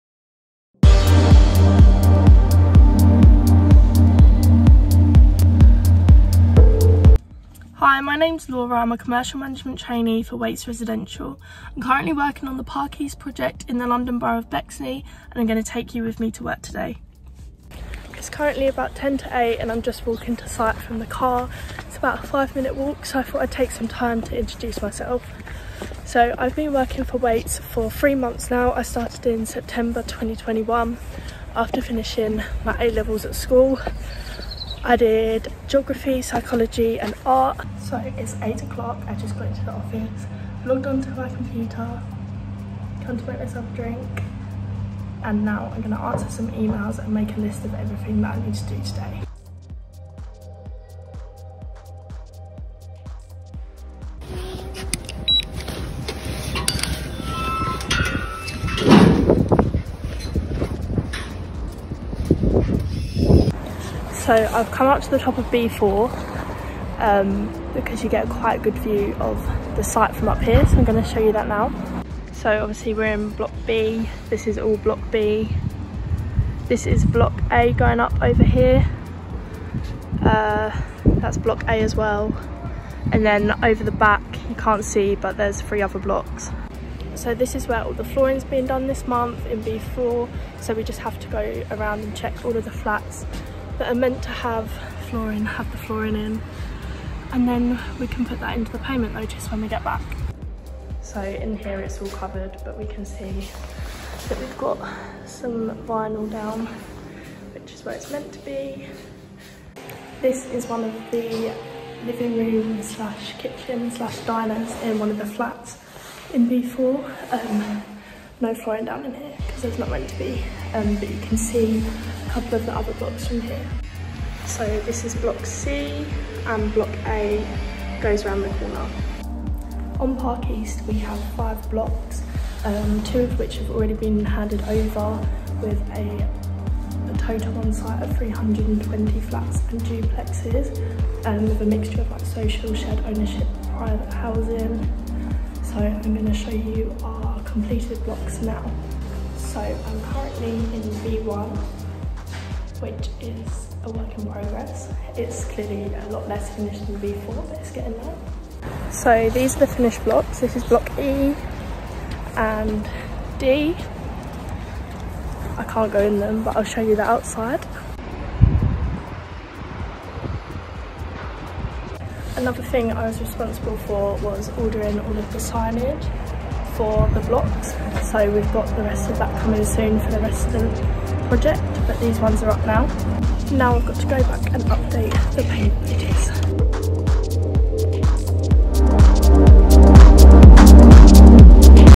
Laura Eves - Commercial Management Trainee
Laura Eves offers us a look inside her daily activities as a Commercial Management Trainee working on Wates Residential’s Park East development in Erith. Take a look at a day in the life for Laura